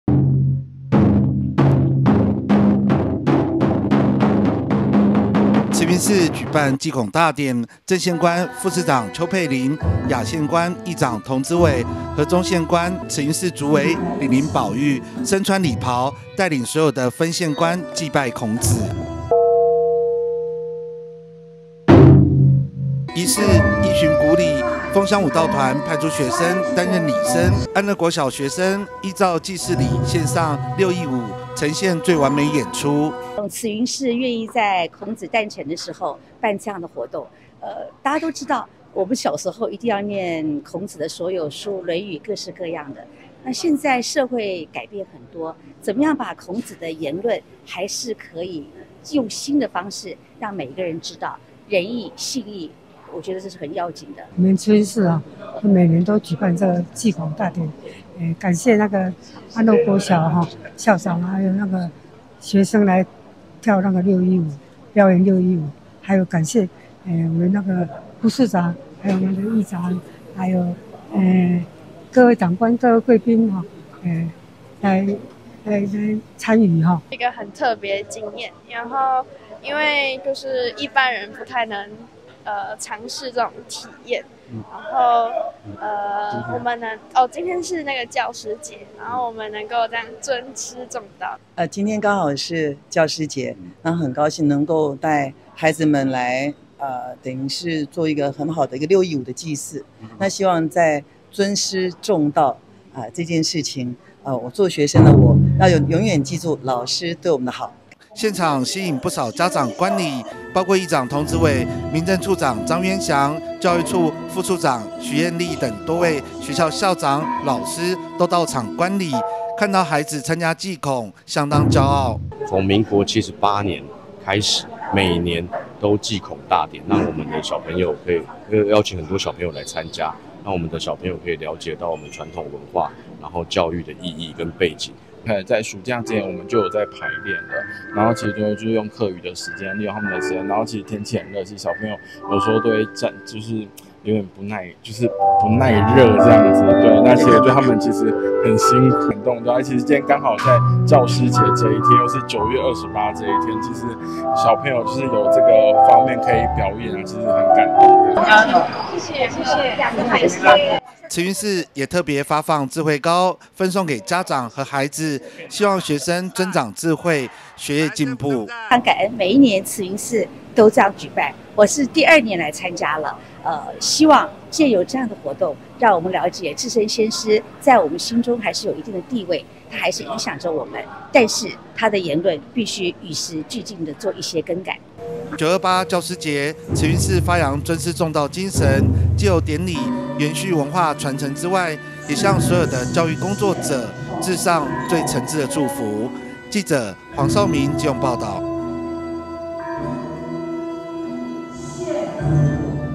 慈雲寺祭孔大典登場 循古禮弘揚尊師重道
安樂國小學生跳六佾舞  邱佩琳.李林寶玉發智慧糕
莘莘學子守護神  慈雲寺護佑學子平安、金榜題名

(中嘉吉隆新聞 記者黃紹銘 /基隆報導) 928教師節，今年是孔子第2574週年的誕辰，為紀念重要節日，基隆慈雲寺舉辦祭孔大典，祭祠流程和獻禮儀式，全都依照古禮進行，安樂國小學童獻上六佾舞，三獻禮，儀式最後副市長邱佩琳與主委李林寶玉、民政處長張淵翔一起發放智慧糕，讓大人小孩一起分享福氣和智慧，儀式莊嚴隆重。
慈雲寺舉辦祭孔大典，正獻官副市長邱佩琳、亞獻官議長童子瑋，與終獻官慈雲寺主委李林寶玉，身穿禮袍，帶領所有分獻官，祭拜孔子。
儀式依循古禮，楓香舞蹈團派出學生，擔任禮生，安樂國小學童也依祭祀禮獻上六佾舞，呈現最完美演出。
現場吸引不少家長，議長童子瑋、民政處長張淵翔、教育處副處長徐嬿立等多位學校校長、老師，看著孩子參與祭孔，相當驕傲，
不只六佾舞熱鬧，慈雲寺特別發放智慧糕，分送給孩子及家長，希望學生增加智慧，學業順利。
928教師節，慈雲寺發揚尊師重道精神，藉由典禮，延續文化傳承之外，也向所有教育工作者致上誠摯的祝福。